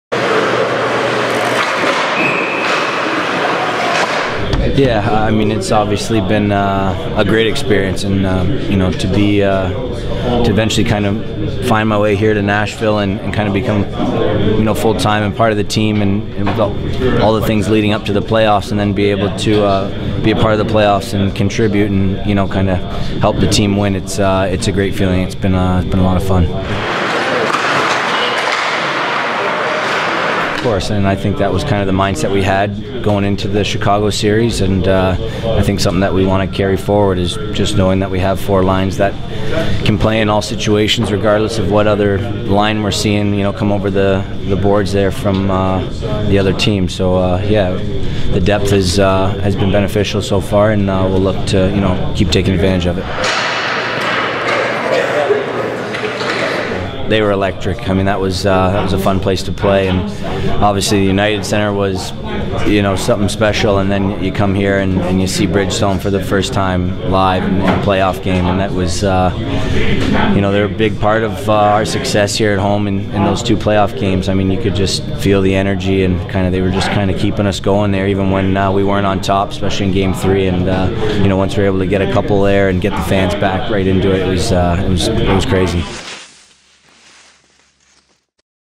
Harry Zolnierczyk finds new hockey life in Nashville
Affectionately known as Harry Z, Harry Zolnierczyk has found new life in the NHL with the Nashville Predators. We caught up with him following practice to talk about his time in Nashville, scoring against Chicago and how much the team feeds off the energy at Bridgestone Arena.